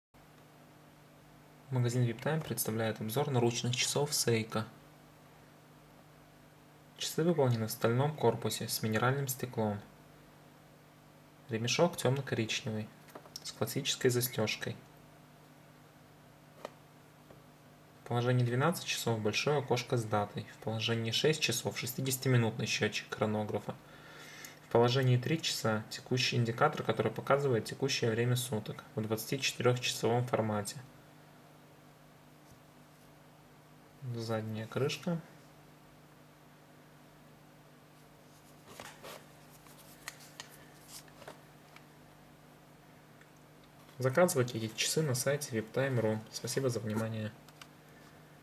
Seiko SPC155P1 обзор от магазина Viptime.ru
Кварцевый хронограф Seiko SPC155P1 c большим окошком показывающим дату в положении 12 часов и хронографом на коже.

Секундная стрелка в положении 9 часов.
В положении 3 часа - 24-часовая шкала суточника.
Аппертура даты типа Большая Дата.
Центральная стрелка - секундная стрелка хронографа. Секундомер с точностью показаний 1/5 сек и максимальным временем измерения - 60 минут. Счетчик часов и минут расположен в положении 6 часов.
Режимы ADD - измерение времени с перерывом и SPLIT (сплит-хронограф) - промежуточный финиш.
Точность хода: не хуже +/-15 секунд в месяц.
Срок службы батареи 5 лет (SR927SW).
Корпус из высококачественной нержавеющей стали.
Диаметр корпуса 42 мм.
Минеральное стекло устойчивое к возникновению царапин.
Водозащита до 10 АТМ.
Кожаный ремешок.